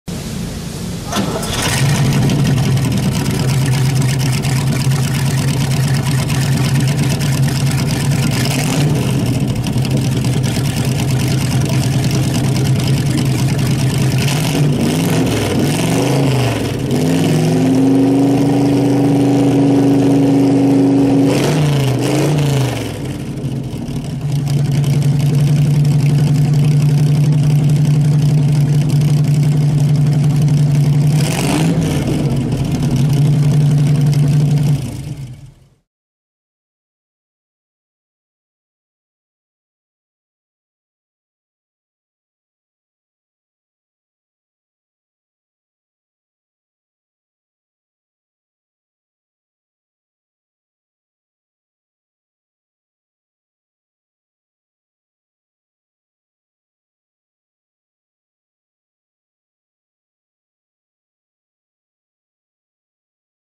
1959 Corvette Stingray Racer Concept Car Revving
The 1959 Corvette Stingray Racer concept car was one of chief designer Bill Mitchell's beautiful one-off concept cars, seen starting up and revving at the Detroit Grand Prix. Thanks to Jacob for starting it up!